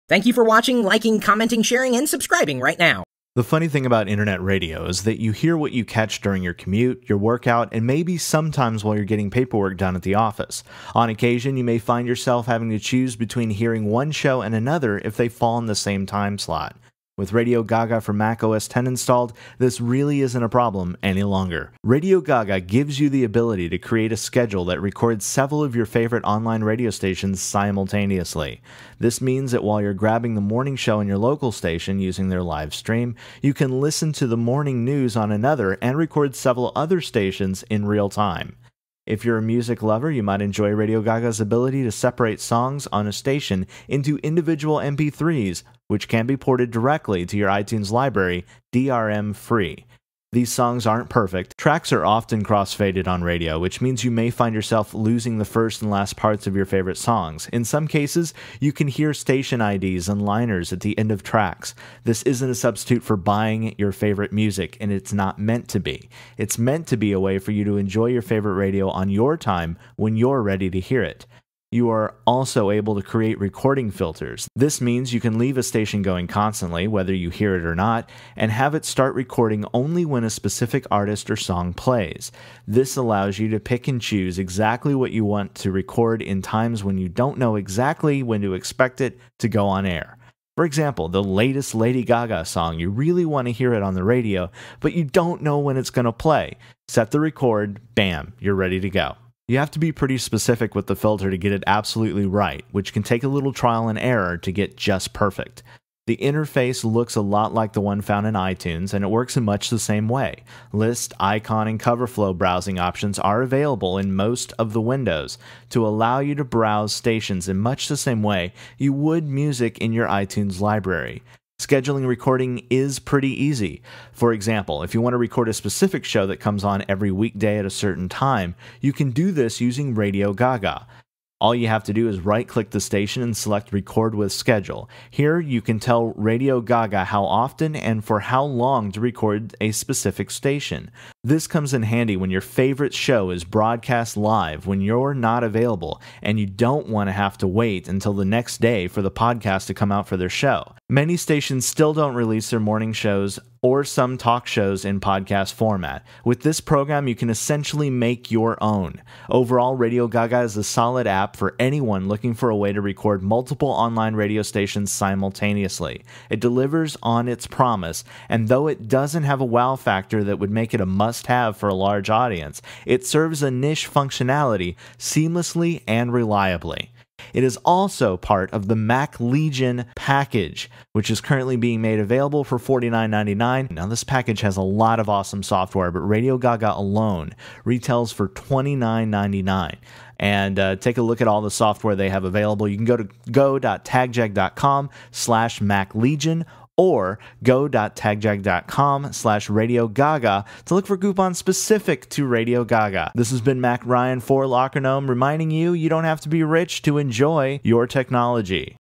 How to Record Internet Radio on the Mac
/how-to-record-internet-radio/ The funny thing about internet radio is that you hear what you catch during your commute, your workout, and maybe sometimes while you're getting paperwork done at the office. On occasion, you may find yourself having to choose between hearing one show and another if they fall in the same time slot. With Radio Gaga for Mac OS X installed, you can record Internet radio easily.